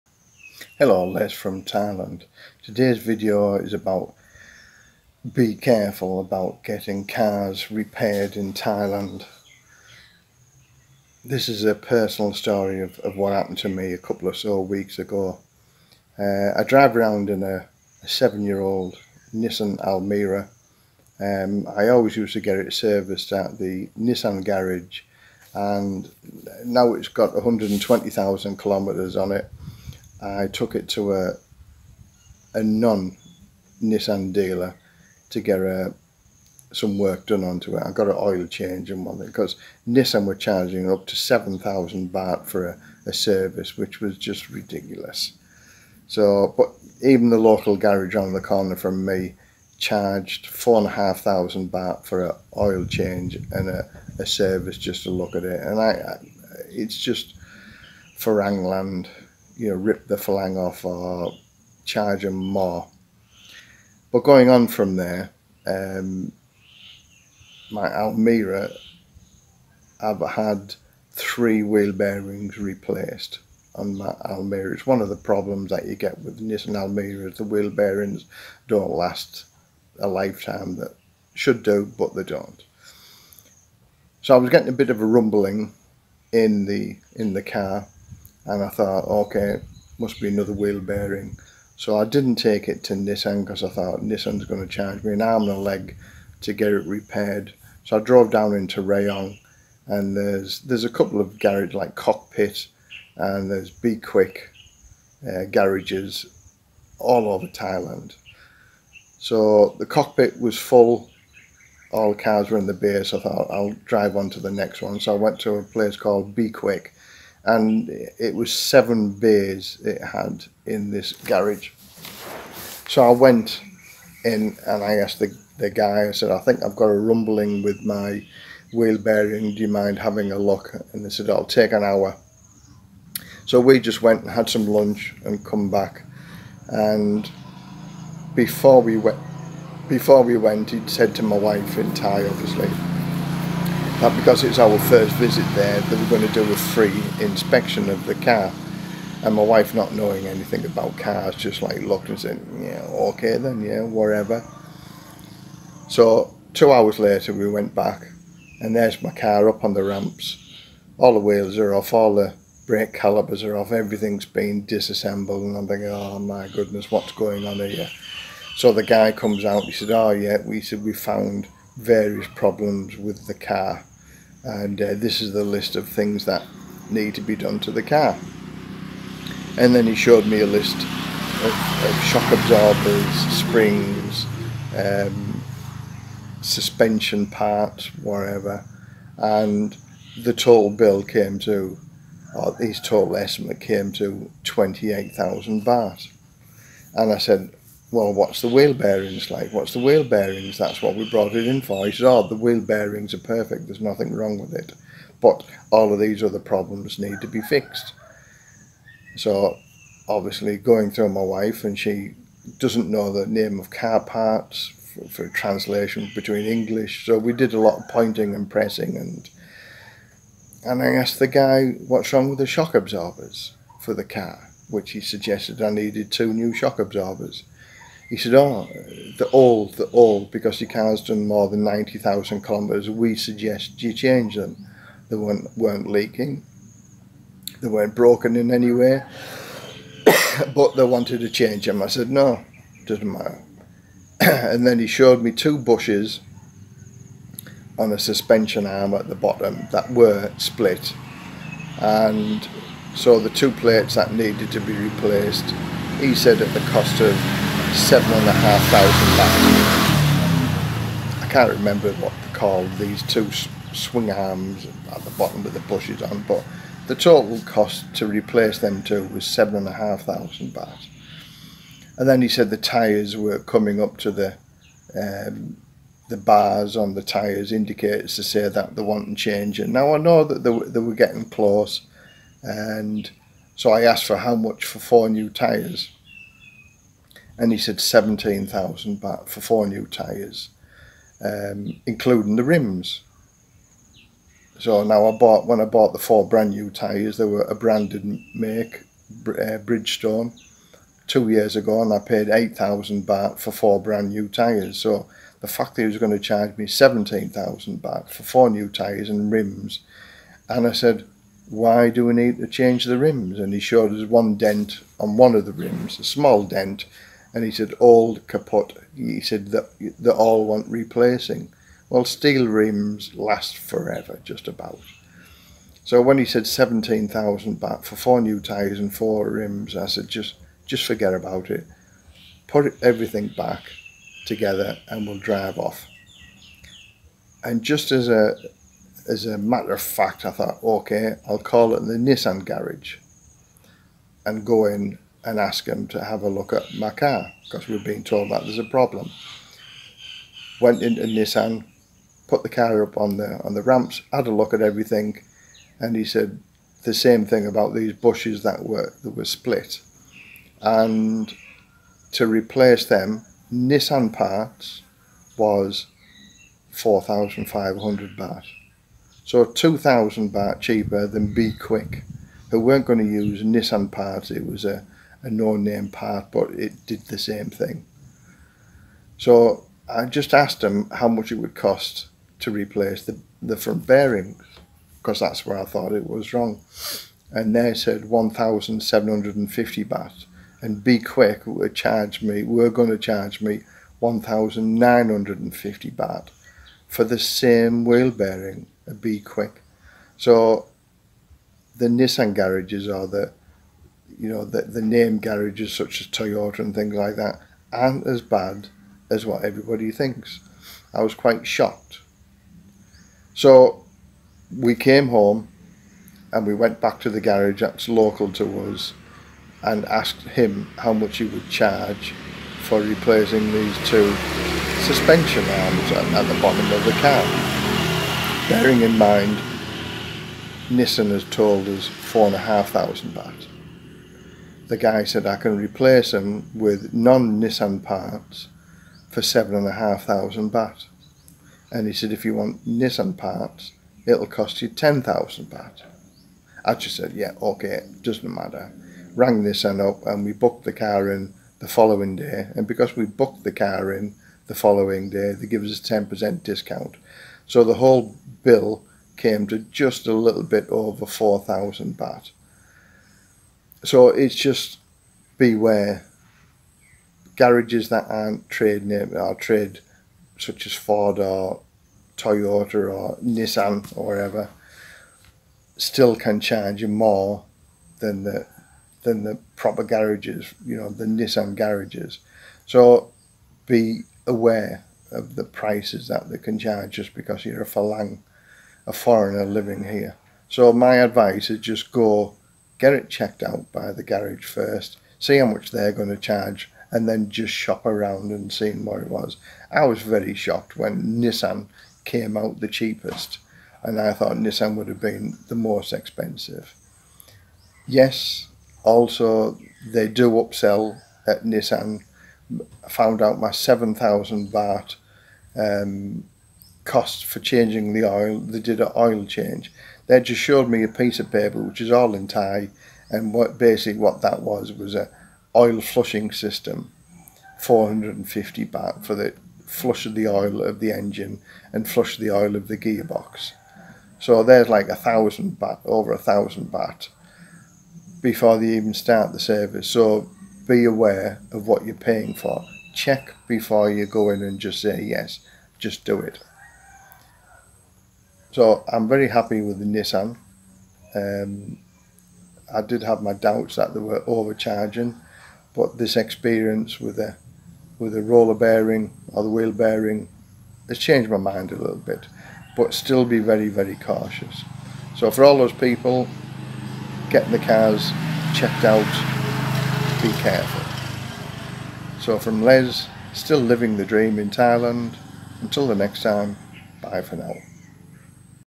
Car service Thailand, Shock result
Car service in Thailand, I was shocked when Nissan were the cheapest had a small repair to do on my Nissan Almera and Nissan were the cheapest by as much as 5,500 Baht on a 4,050 baht repair. I tried 3 different garages for the same repair and was quoted some silly prices.